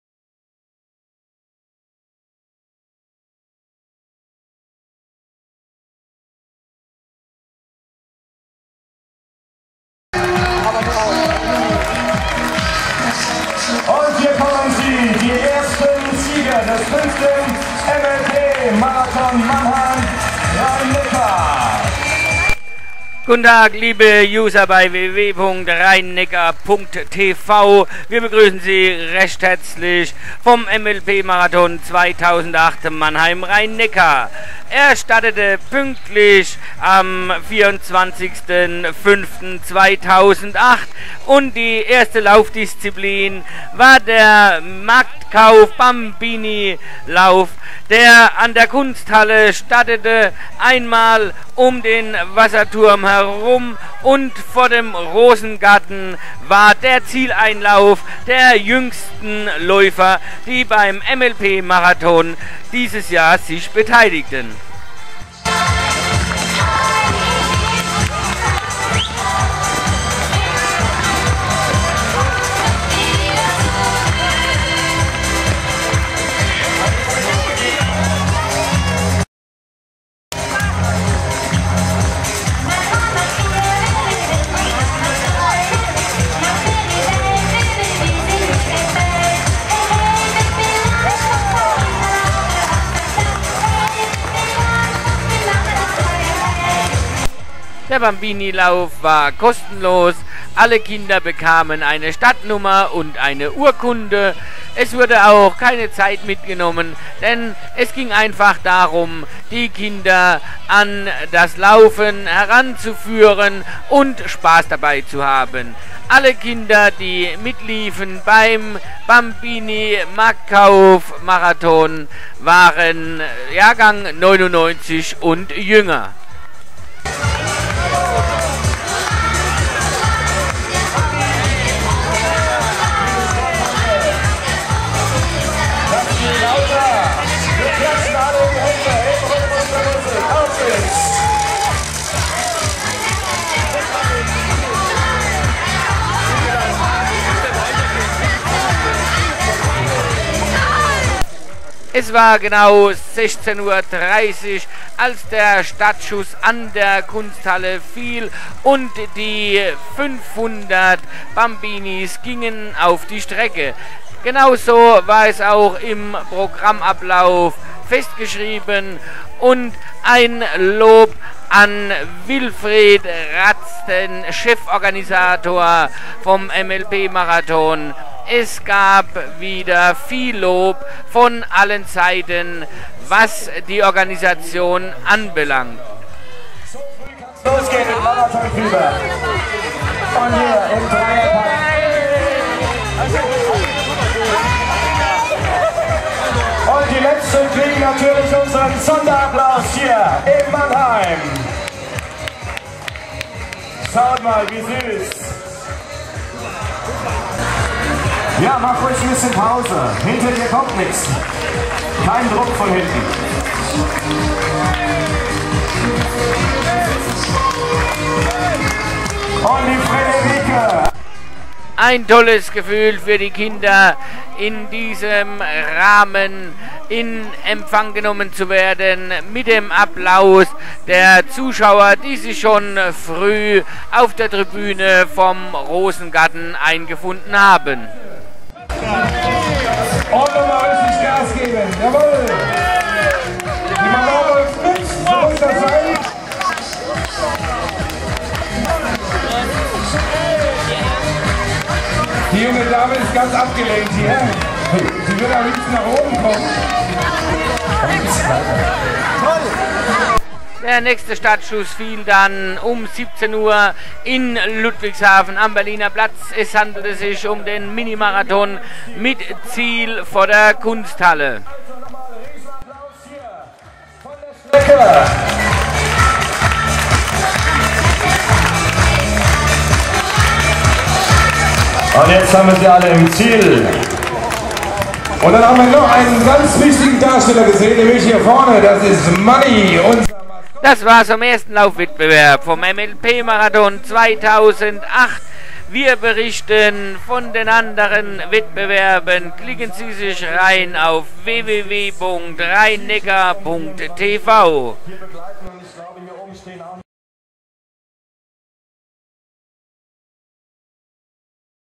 MLP Marathon 2008 Zieleinlauf Bambini
Aus der Metropolregion Rheinneckar präsentiert den Zieleinlauf der Bambinis vom MLP Marathon Mannheim Rheinneckar 2008